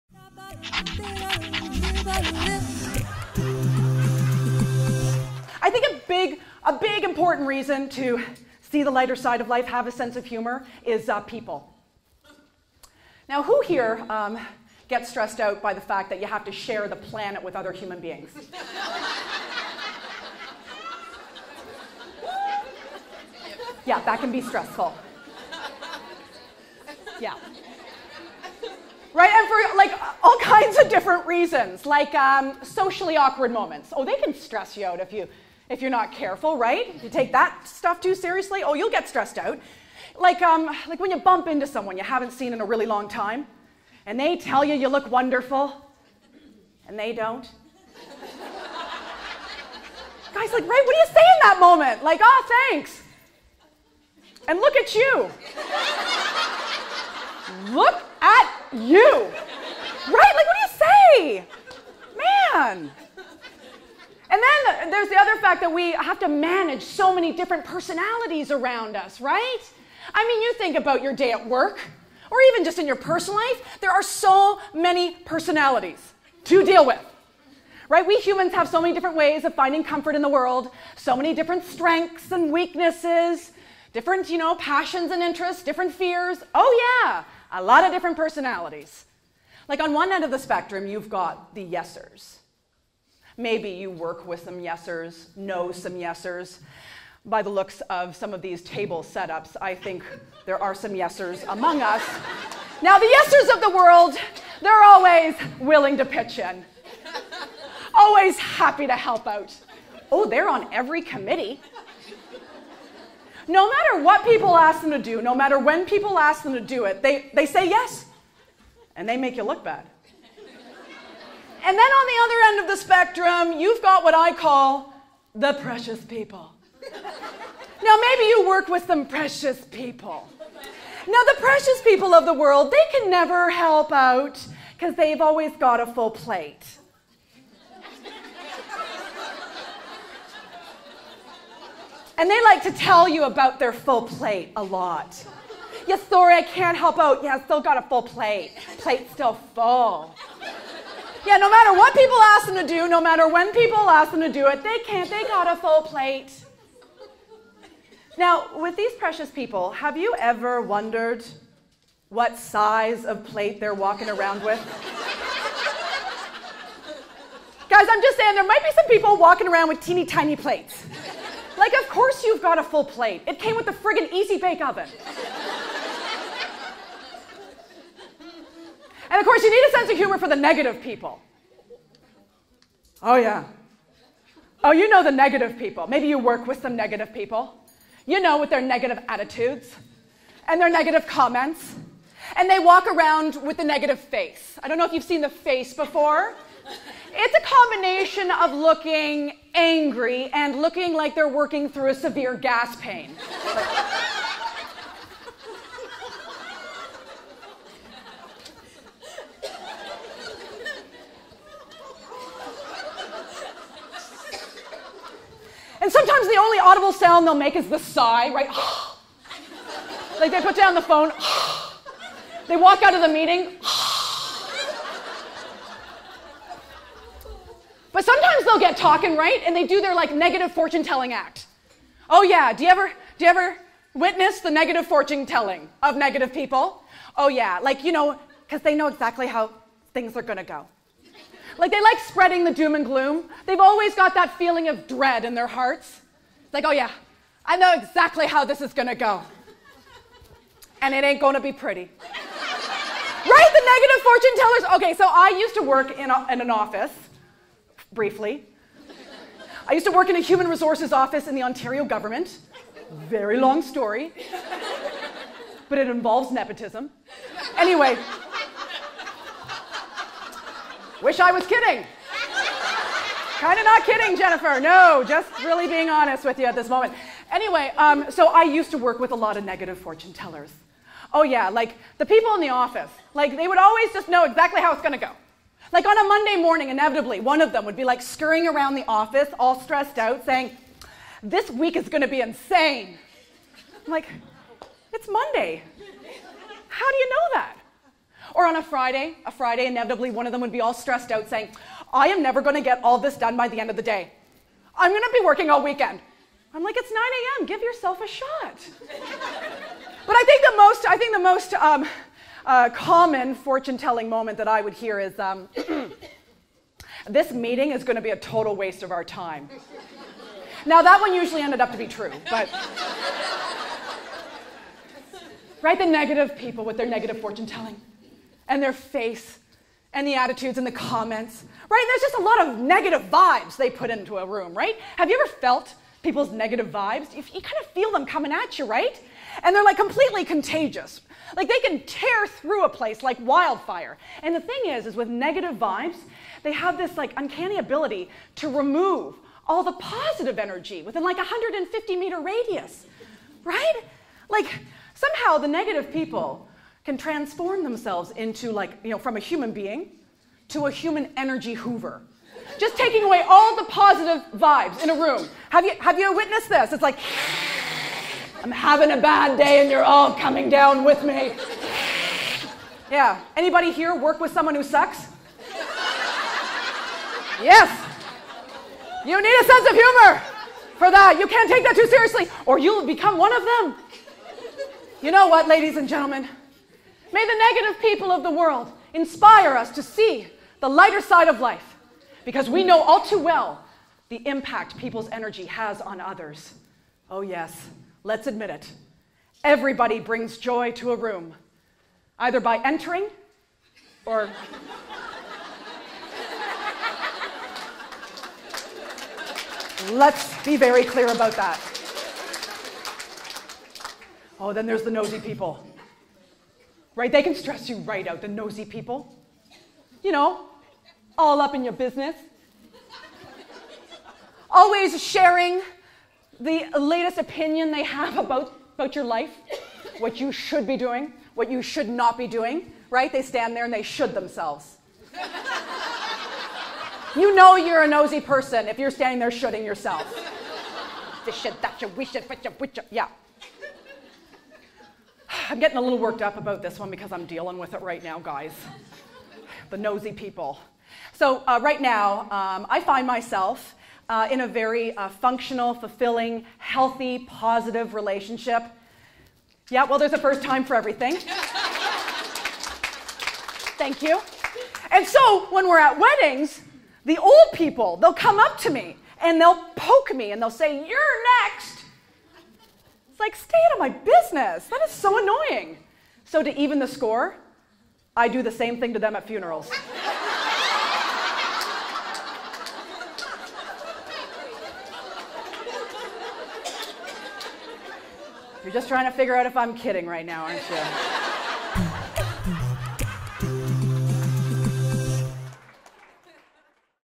Susan Stewart - The Various Way People Can Stress You Out
Comedian/author/speaker, Susan Stewart, shares the various reasons why it's important not to take people too seriously.